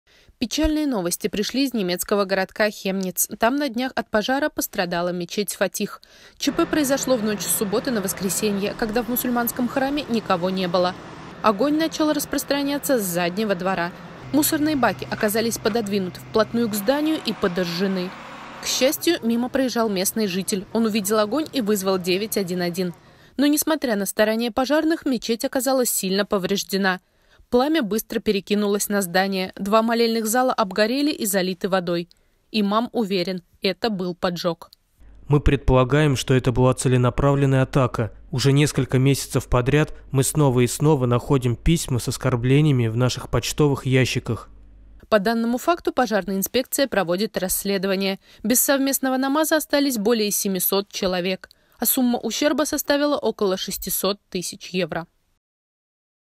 Подожгли мечеть! 700 человек пострадали!!
Пододвинули баки к двери и подожгли? Если б не случайный очевидец, мечеть сгорела бы дотла. Подробности – в сюжете.
Оказать поддержку каналу НУР ТВ можно по реквизитам:
Карта Сбербанка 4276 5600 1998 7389 (MARLEN MAKHMETOV)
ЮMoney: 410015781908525